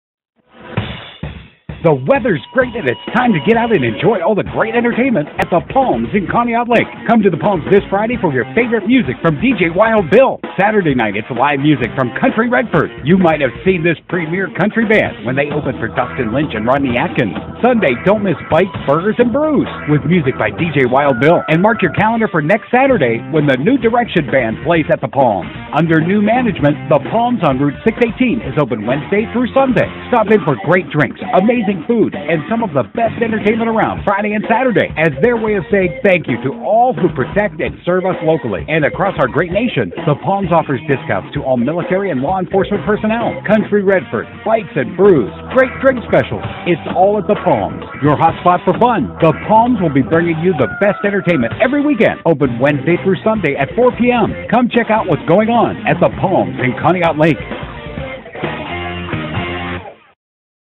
CR the palms july 16, 2016
Country Redford at The Palms in Conneaut Lake, PA. Saturday, July16th, 2016 @ 9pm